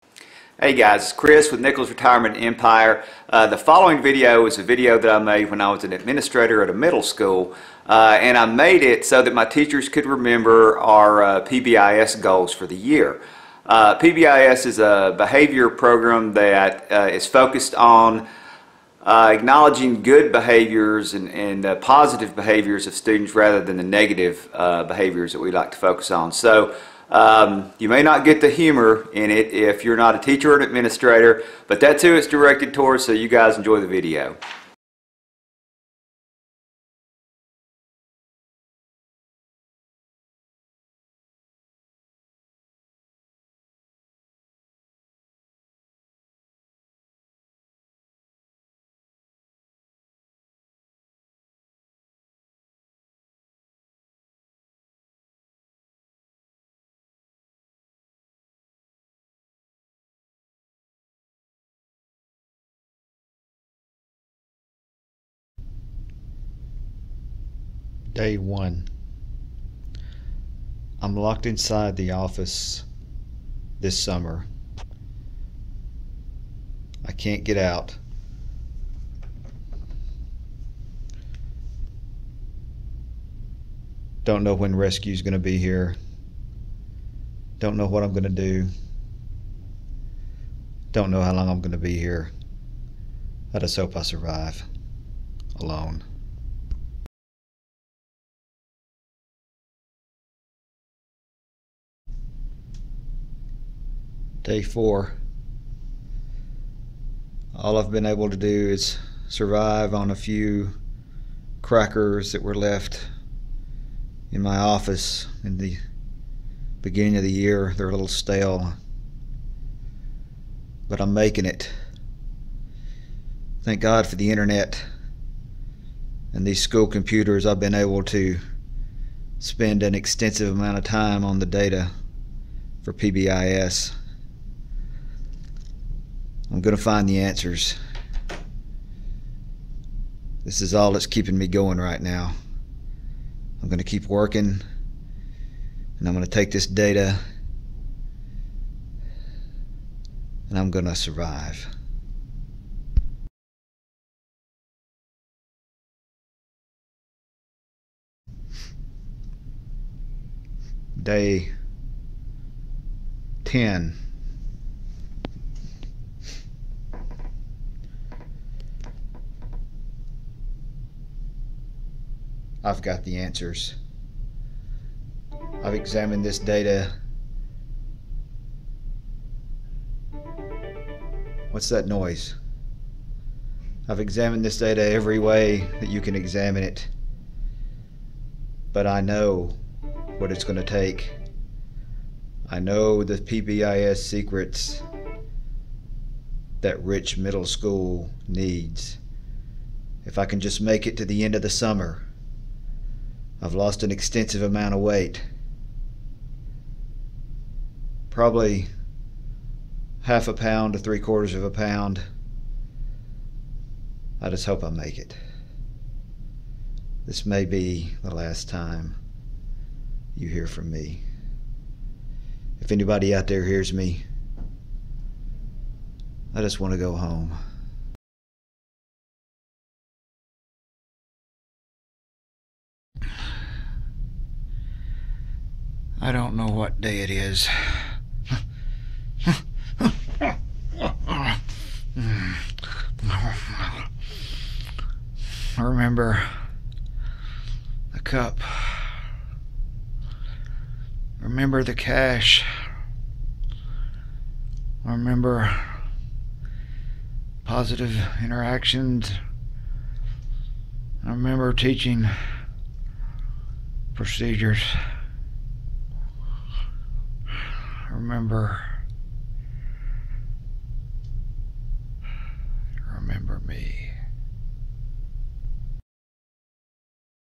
PBIS Data Video for Administrators and Teachers-Nichols Retirement Empire
PBIS Data Video for Administrators and Teachers-Nichols Retirement Empire. This video was a pre-planning video I made for the faculty of my Middle School to introduce the teachers to the PBIS videos for the year. Using Positive Behavior Interventions and Supports (PBIS) we were able to cut out discipline referrals by more than half over a short two year period. We had a good time making funny videos for the faculty and students.